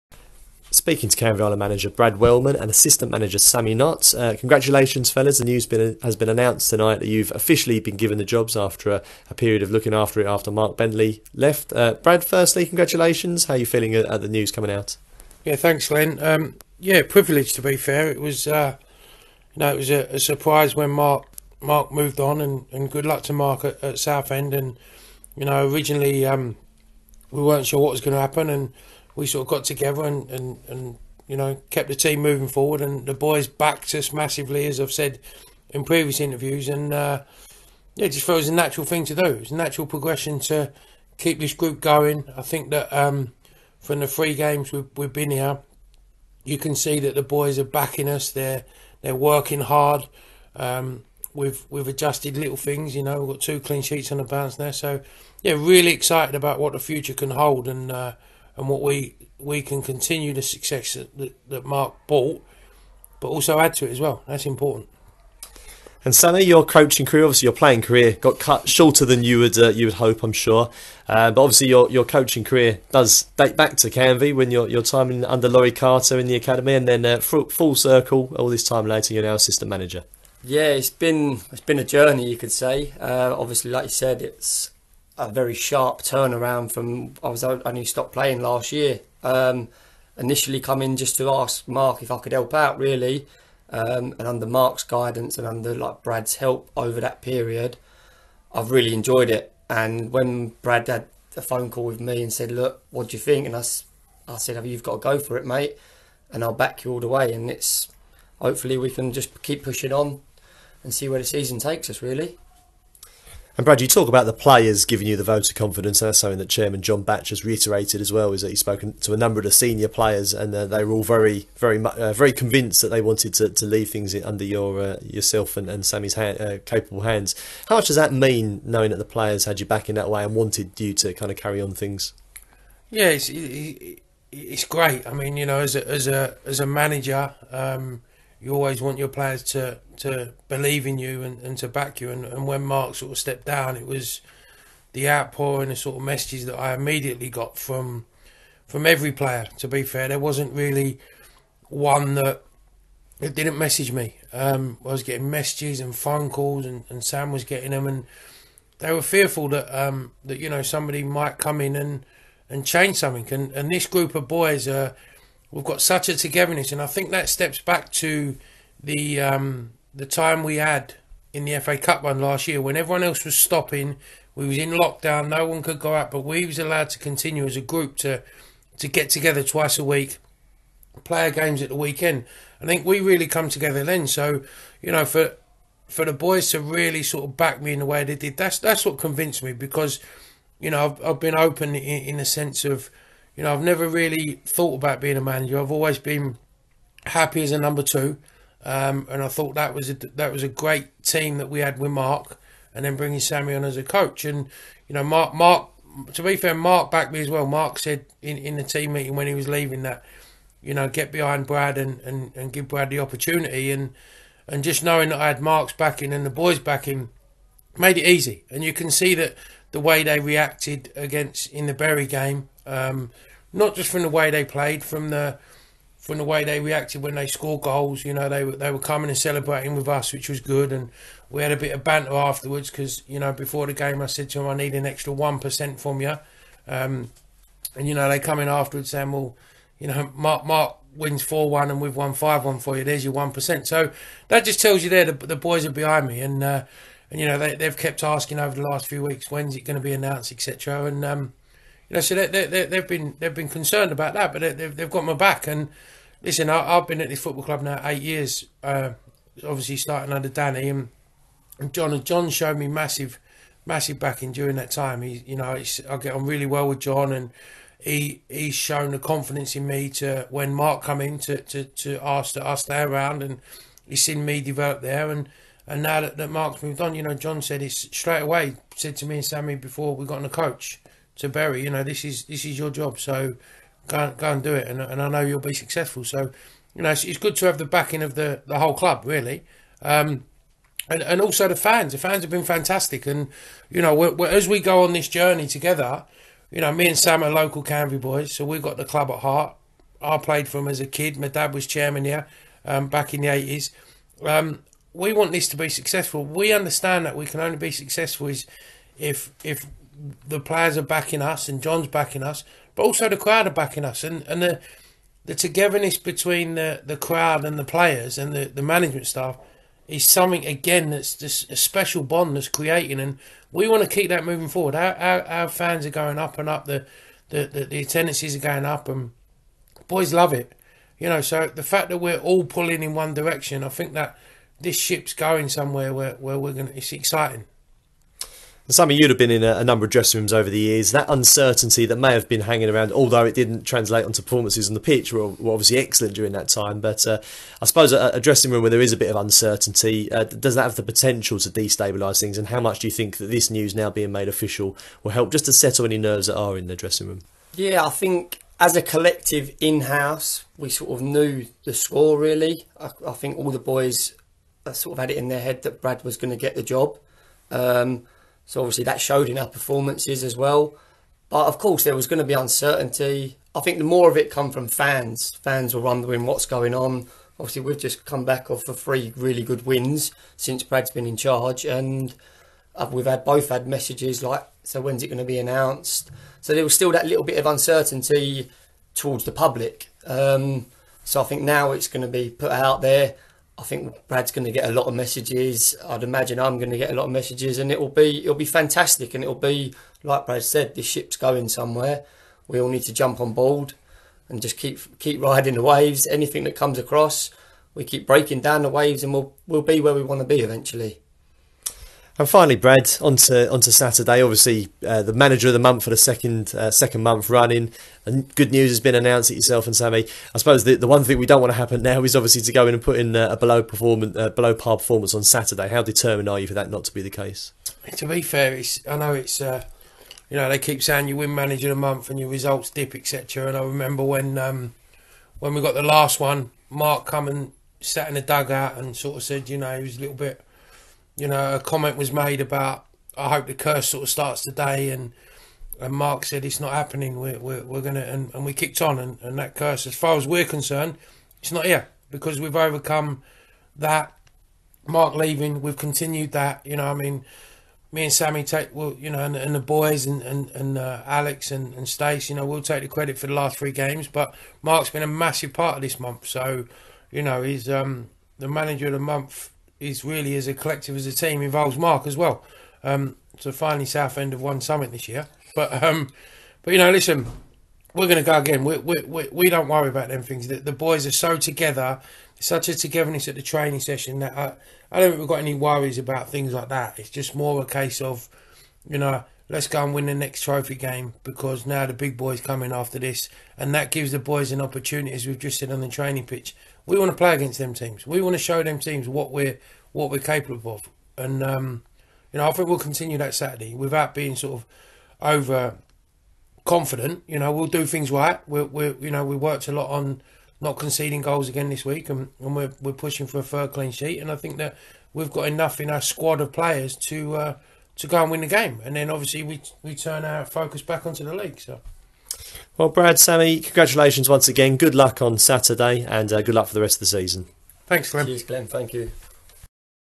Brad Wellmen & Sammy Knott Interview - Thursday 11th November 2021
New Canvey Island manager Brad Wellmen and assistant manager Sammy Knott speak following them officially being named as the club's new management team.